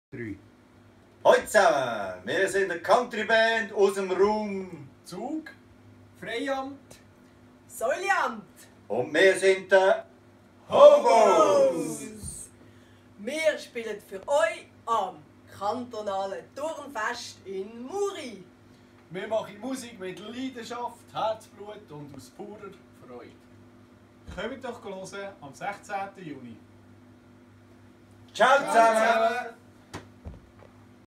The Hobos - live am KTF17!
The Hobos trättet am Fritig 16. Juni 2017 bi eus im Sieger-Kafi uf - do muesch ombidengt debi sii! ktf17.ch